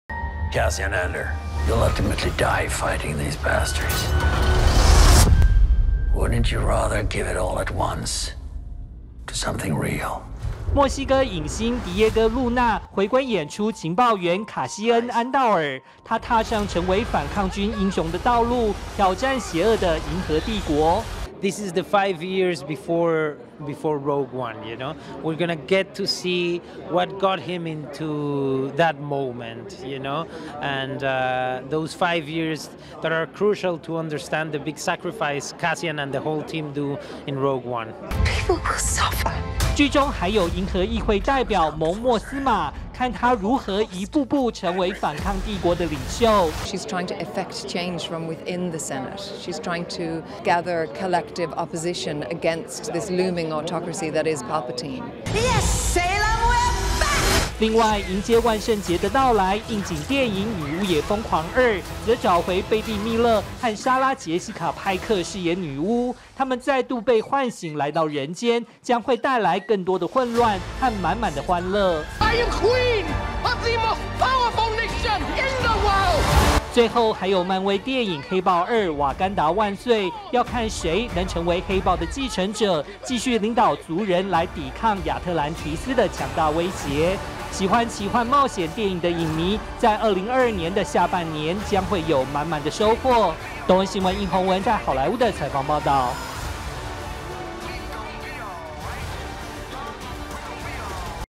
"星際大戰外傳：俠盜一號"前傳影集"安道爾" 俠盜蛻變成為一名反抗軍英雄
「星際大戰」迷，有福了！「星際大戰外傳：俠盜一號」的前傳影集「安道爾」，要帶影迷來見證一位俠盜，如何蛻變為一名反抗軍英雄。請看 東森新聞記者，因宏文，來自好萊塢的 獨家採訪報導。

墨西哥影星「迪耶哥路那」回歸演出情報員「卡西恩安道爾」。他踏上成為反抗軍英雄的道路，挑戰邪惡的銀河帝國。

劇中還有銀河議會代表「蒙茉斯瑪」，看她如何一步步成為反抗帝國的領袖

另外，迎接「萬聖節」的到來，應景電影「女巫也瘋狂2」，則找回「貝蒂蜜勒」、和「莎拉潔西卡派克」飾演女巫。她們再度被喚醒來到人間，將會帶來更多的混亂和滿滿的歡樂。

最後，還有漫威電影「黑豹2：瓦干達萬歲」，要看誰能成為黑豹的繼承者，繼續領導族人來抵抗「亞特蘭提斯」的強大威脅。

喜歡奇幻冒險電影的影迷，在2022的下半年，將會有滿滿的收穫。